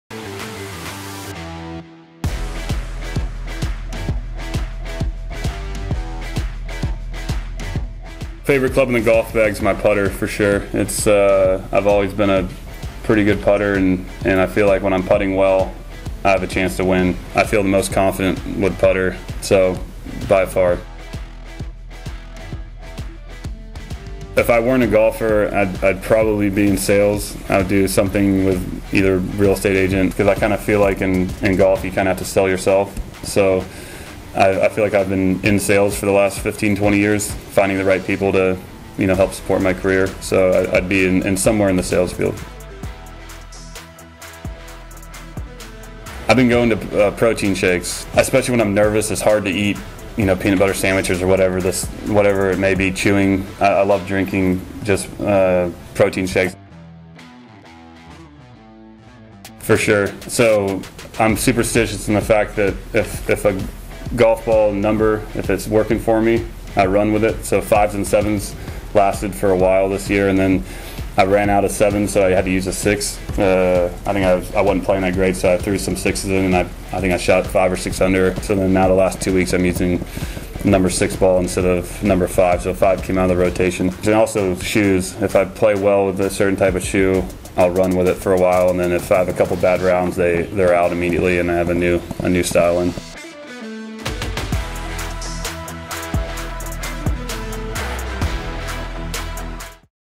Quick Hits with Lanto Griffin
Golfweek's Steve DiMeglio talks with PGA Tour winner, Lanto Griffin, about his favorite club in the bag, what his job what be if he weren't a golfer, and much more.